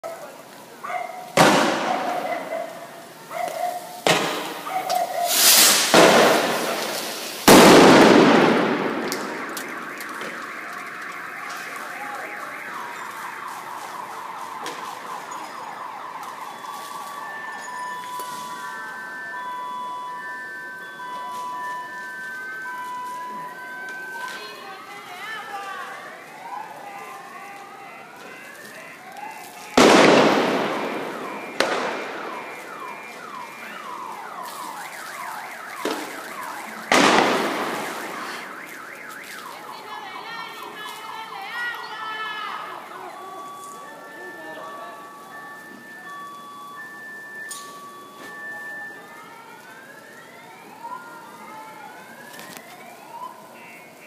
Lacrimogenas en Chacao martes 11/03/2014
Equipo antimotín de la Guardia Nacional intenta disolver manifestación con bombas lacrimógenas. Imágenes calle Sucre de Chacao. Video José Miguel Chirino